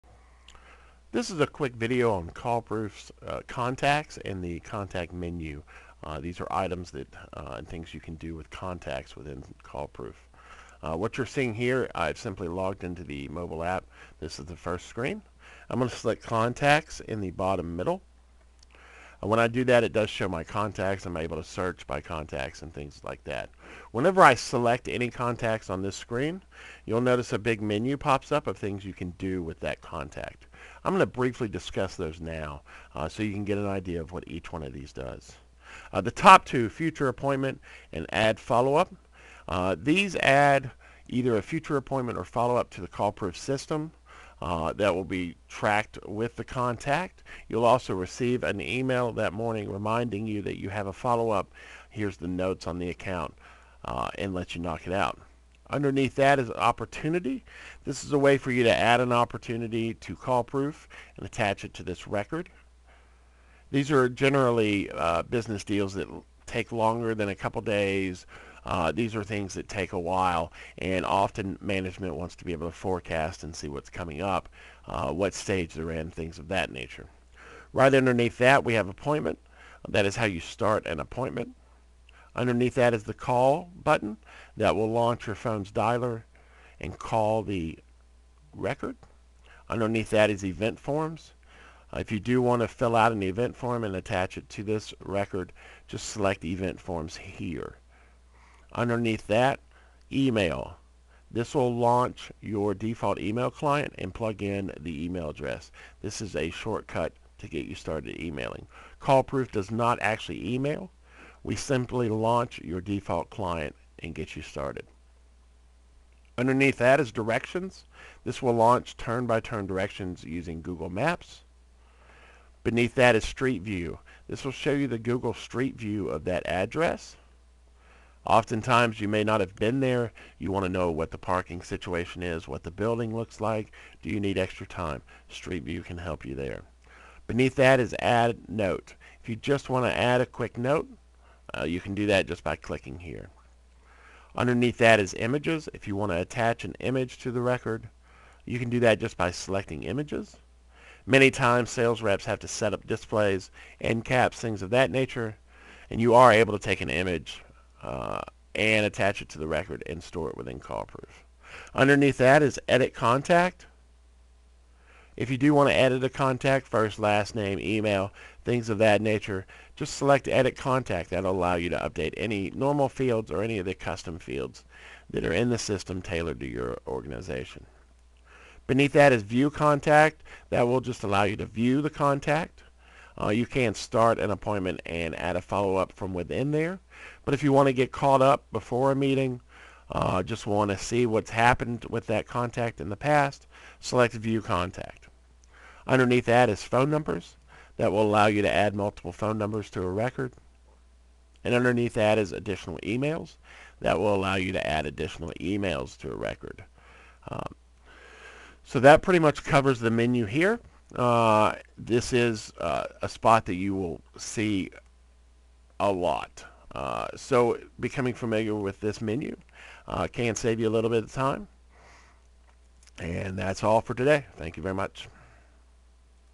Contact Features
Accessing contacts in CallProof mobile app is very easy.  This video shows you the many actionable feataures when selecting a contact, from make a call, sending an email, making an appointment, directions, street view, and many more great features to help you manage your customers and prospects.